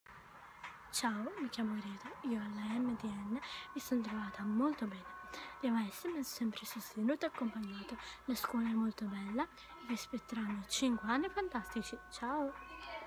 Greta B.
I ragazzi di quinta hanno lasciato un video-messaggio di augurio e incoraggiamento per l'inizio dell'anno scolastico ai bambini delle future prime.